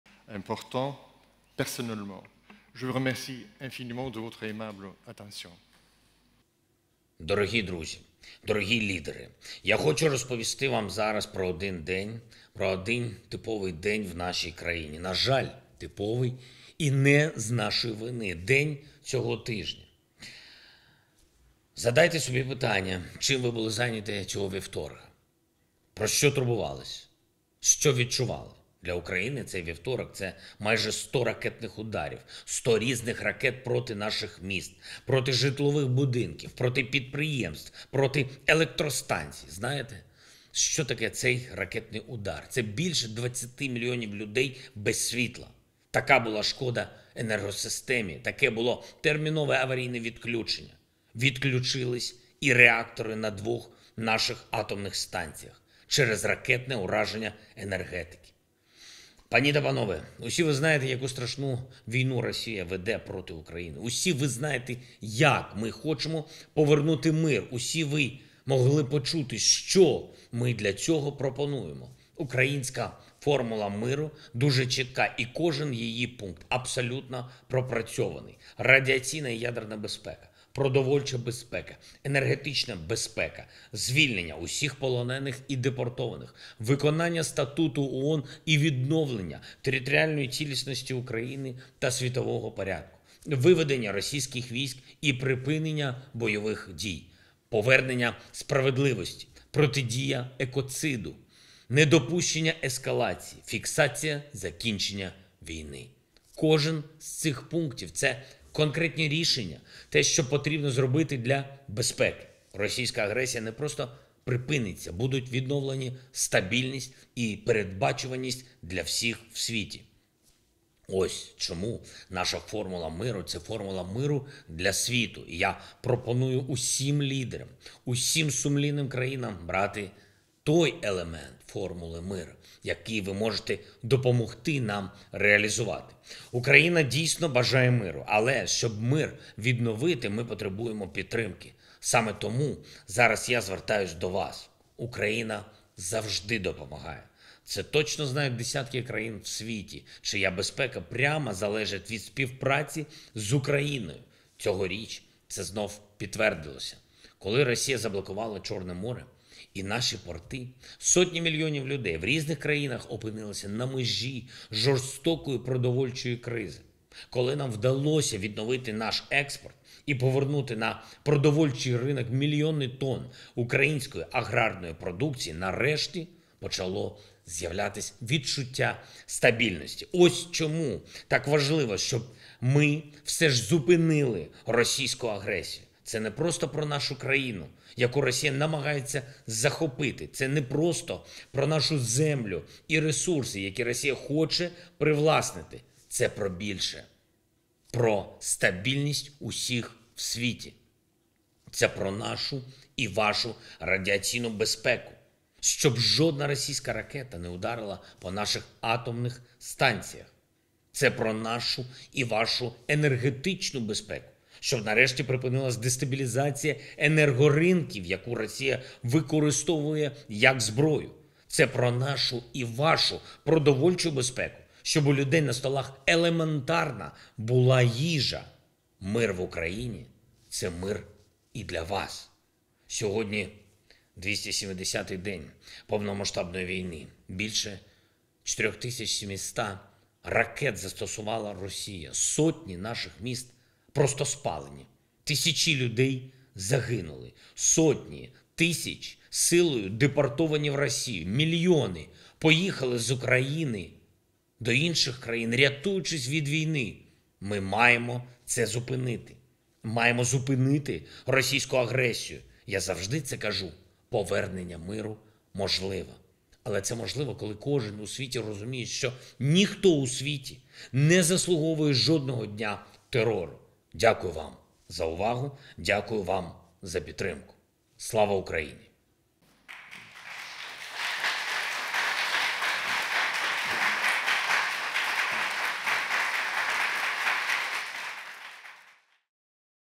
З 24 лютого по Україні випустили 4700 РОСІЙСЬКИХ РАКЕТ — Зеленський
«Українська формула миру дуже чітка – і кожен її пункт абсолютно пропрацьований. Радіаційна і ядерна безпека. Продовольча безпека. Енергетична безпека.

Звільнення всіх полонених і депортованих. Виконання Статуту ООН і відновлення територіальної цілісності України та світового порядку. Виведення російських військ і припинення бойових дій. Повернення справедливості. Протидія екоциду. Недопущення ескалації.

Фіксація закінчення війни. Кожен із цих пунктів – це конкретні рішення, те, що потрібно зробити для безпеки. Російська агресія не просто припиниться – буде відновлено стабільність і передбачуваність для всіх у світі. Ось чому наша формула миру – це формула миру для світу. І я пропоную всім лідерам, усім сумлінним країнам обрати той елемент формули миру, який ви можете допомогти нам реалізувати».

Президент звернувся до міжнародної організації співробітництва франкомовних країн світу Франкофонія. Вона об’єднує понад 50 держав, а також 27 держав є її членами-спостерігачами.